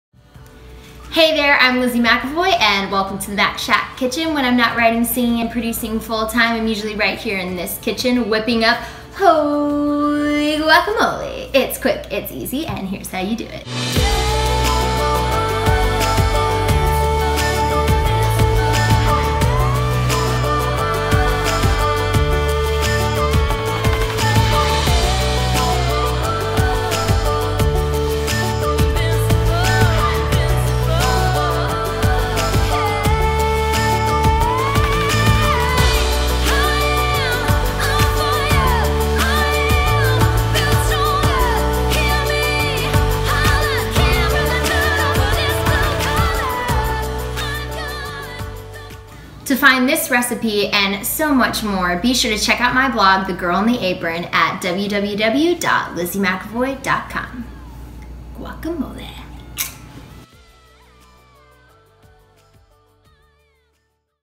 Holy Guacamole!- The Girl In The Apron- Lizzy McAvoy
-HOLY GUACAMOLE!- The Girl In The Apron

Note from The Girl in The Apron:

I never used to be a guacamole fan seeing that I could never eat raw tomatoes. A few of my friends were not fans either since they couldn't eat raw onions. Seeing that avocados are so good for you, I felt like we were missing out on a healthy snack alternative. So one Pina Colada Girls Night, I decided to lose the tomato and the onion but not lose in the flavor department. TAH DAH! Holy Guacamole was born. Packed full of spices and fresh cilantro this guacamole has become a staple in my house and frequently requested among my friends, even the tomato/onion loving ones:) Delicioso!
HAPPY COOKING!

Ingredients:

Yields 4-5 Cups

6 fresh avocados, skin removed, pitted and diced
Juice of 1 Lime
1 cup chopped fresh cilantro
2 teaspoons cumin
1 teaspoon garlic powder
1 teaspoon onion powder
1 teaspoon salt
1 teaspoon ground black pepper
Garnish with Paprika, Spring of Cilantro and a Slice of Lime

Here’s How You Do It:

Slice the avocado open vertically. Using a towel to cover your hand, remove the pit. Using just the tip of the knife, slice into the avocado flesh, making sure to not pierce the skin. Follow the natural curve of the avocado using the tip of the knife. Repeat until you have cut through the entire avocado vertical and horizontal, to make cubes.

Gently work a spoon between the flesh and the skin of the avocado. Following the curve of the avocado with the side of the spoon.

Place your cubed avocado into a large bowl. Add the lime juice, cumin, garlic powder, onion powder, salt and pepper. With a fork, mash together the avocado and the spices to desired consistency.

Add the fresh cilantro and mix well. Garish with a lime slice, paprika for color and sprig of cilantro. Serve with a side of your favorite chips.